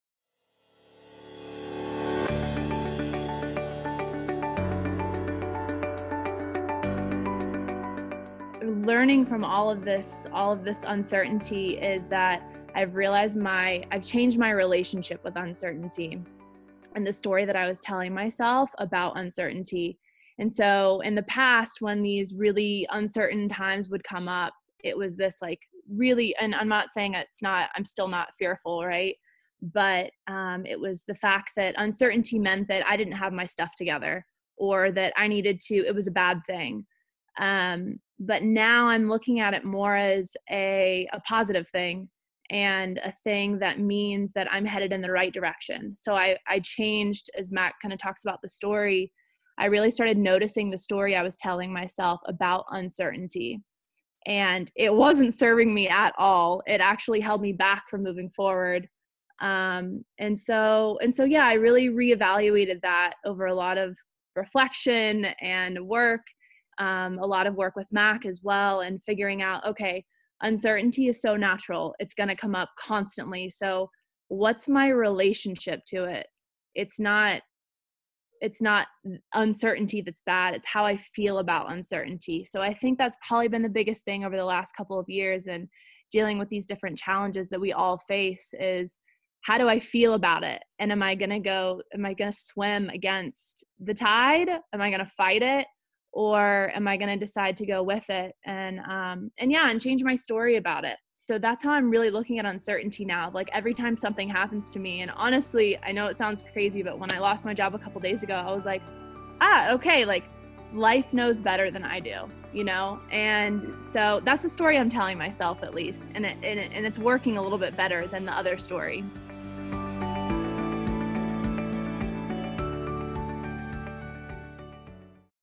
Bishop Reid~ Changing Our Relationship with Uncertainty
Uncertainty is often used as a negative term. How can we change our relationship with uncertainty and how we feel about it? Here is some insight from Bishop Reid during our momentofimpact online conversation!